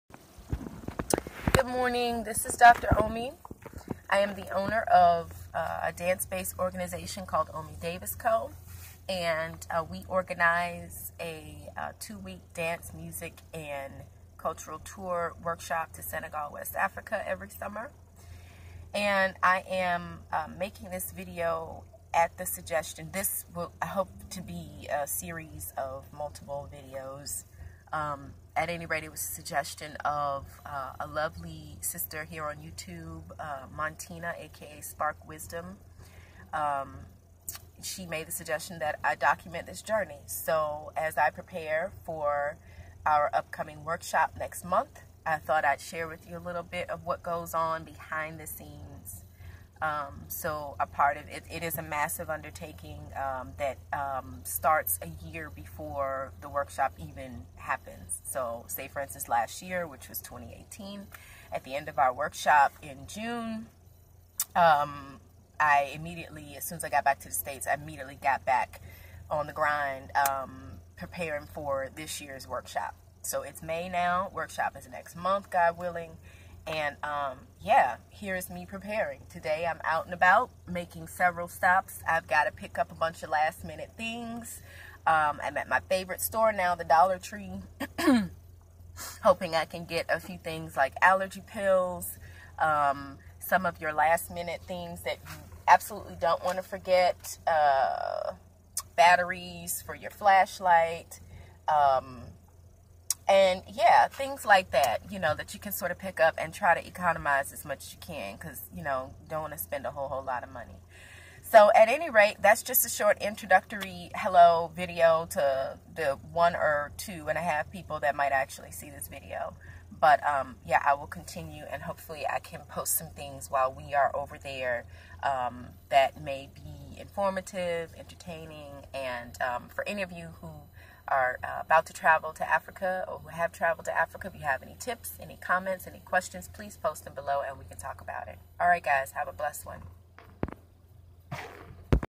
Intro and Preparing for Workshop in Senegal, West Africa
Hey guys! This is a short intro to me and my dance organization, omi davis co. We organize a two week dance, music and cultural tour workshop in Senegal every year. As I prepare for this year's trip, I'm bringing you along with me!

Danceinafrica.com

Shout out to Montina AKA SPARKWISDOM for suggesting that I document this journey.